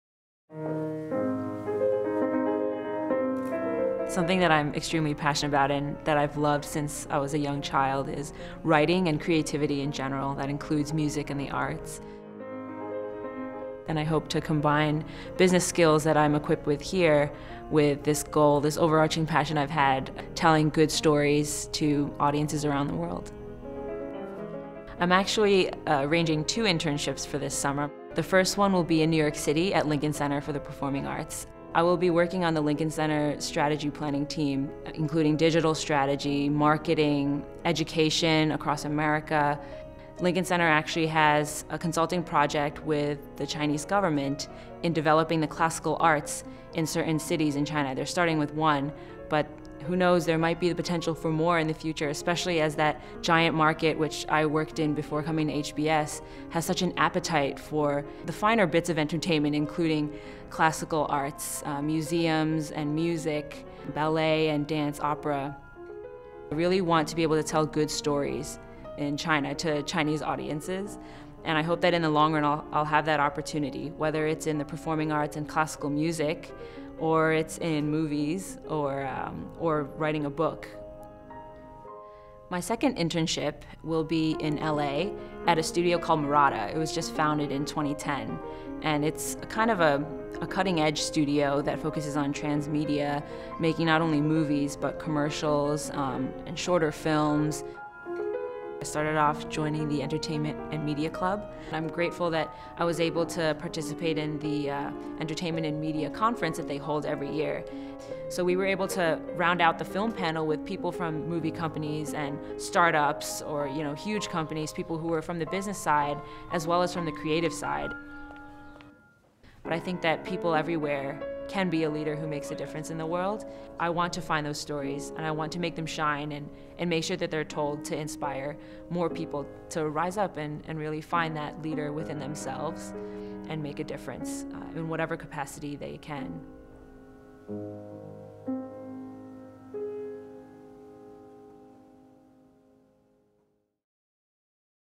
Ann Chao, MBA 2013
Ann Chao talks about her experience at HBS and its impact on her career path.